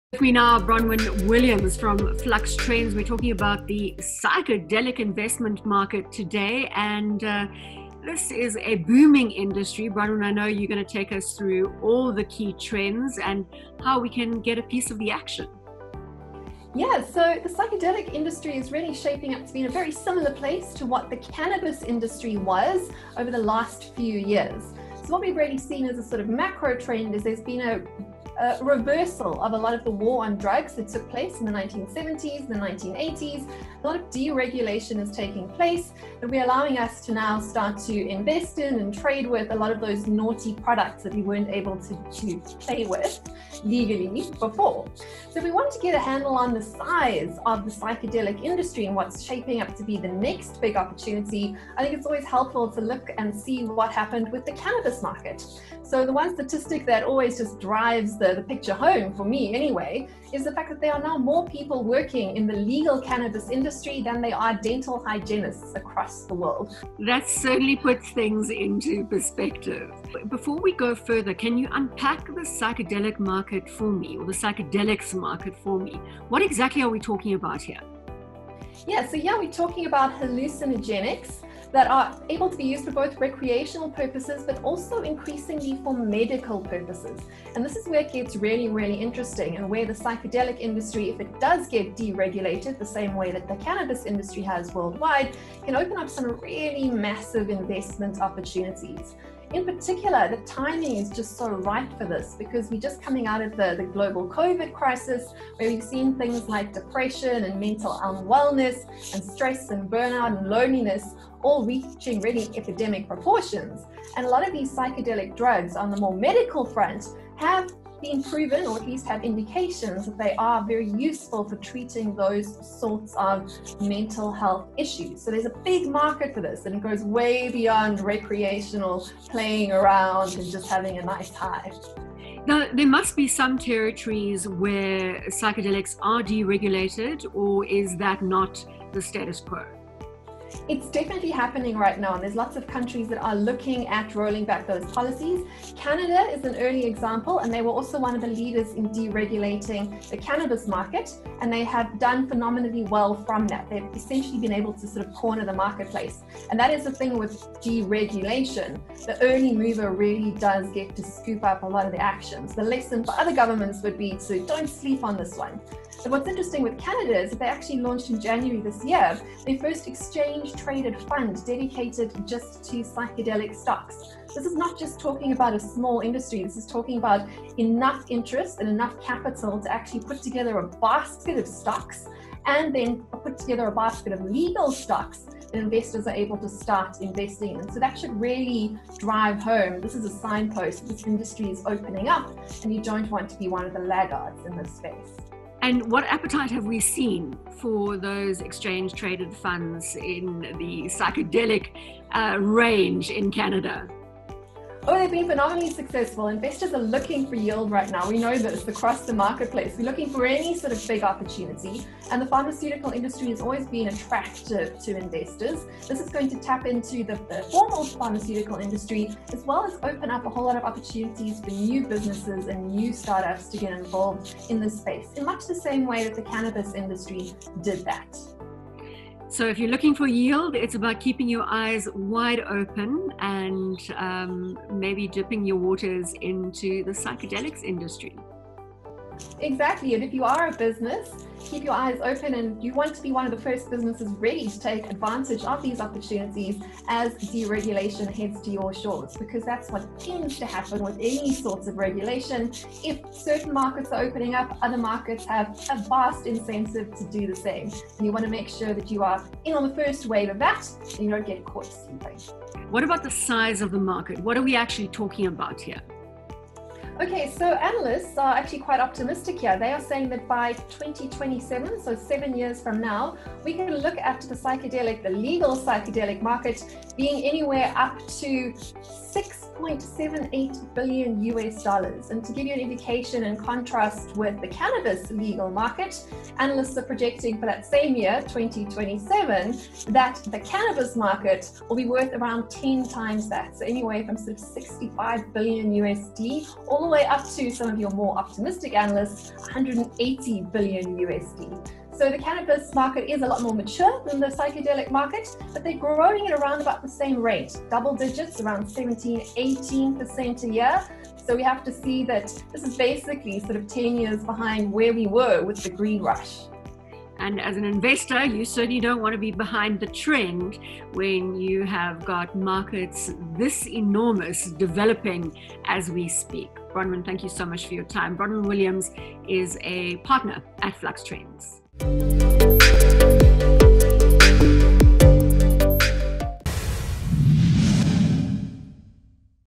Future Focus Searching for Yield? Keep an eye on The Psychedelic market.
The first Psychedelics ETF has officially launched on in the Canadian market. Predictions are that by 2027 the burgeoning Psychedelic market could be worth a whopping $6bn making it an investment opportunity that may be well worth exploring as the market starts to deregulate.